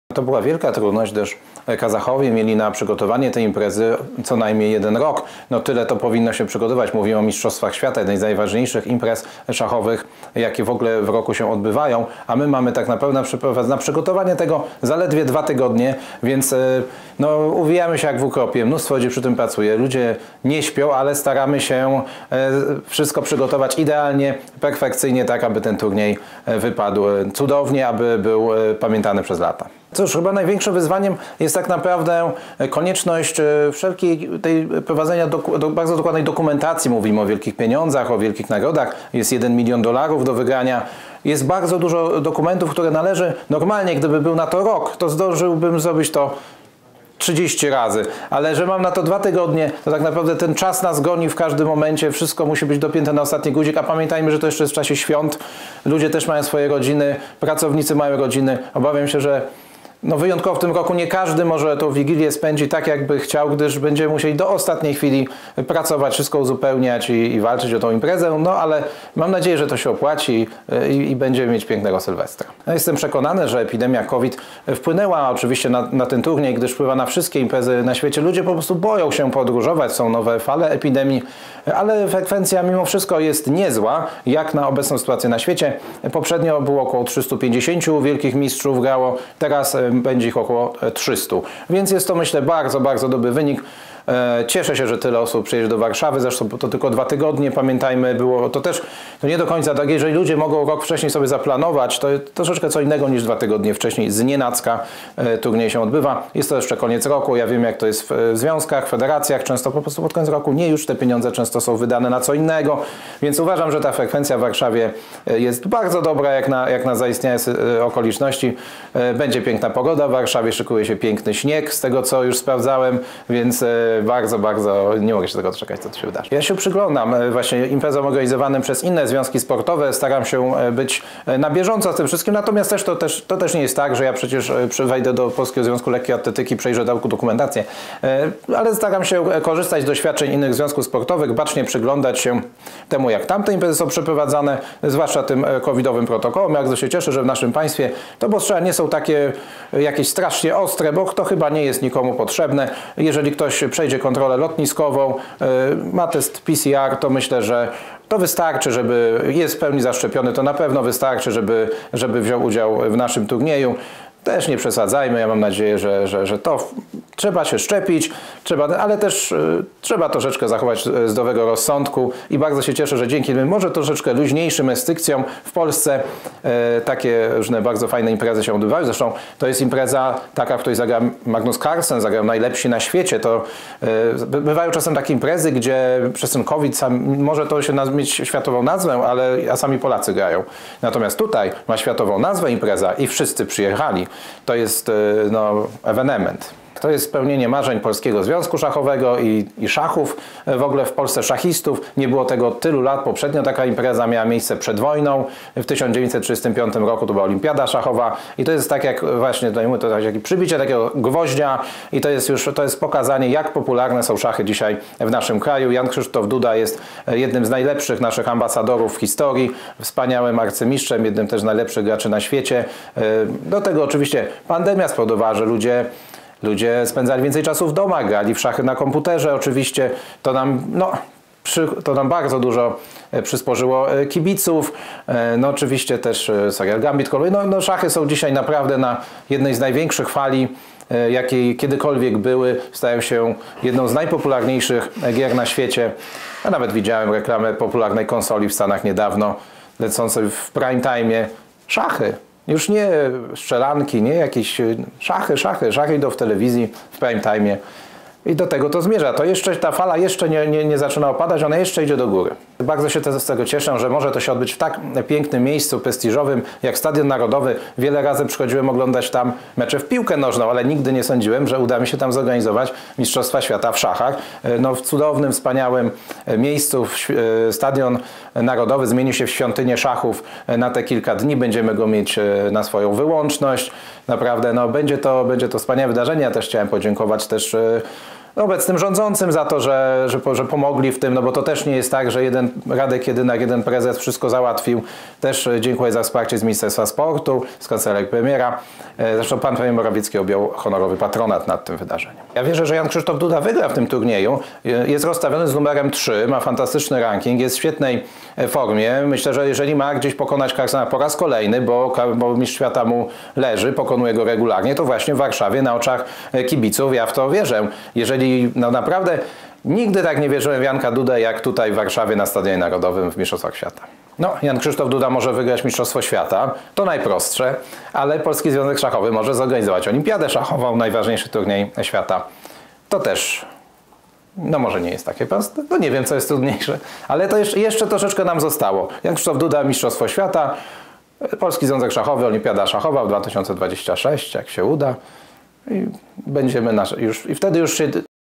Radosław Jedynak - Mistrzostwa Świata w szachach Warszawa 2021
– Wierzę, że Jan-Krzysztof Duda wygra w tym turnieju, ma świetny ranking, jest rozstawiony z numerem trzecim. Gorąco w niego wierzę, że przed własnymi kibicami pokaże się z jak najlepszej strony i zwycięży – mówi prezes Jedynak. Więcej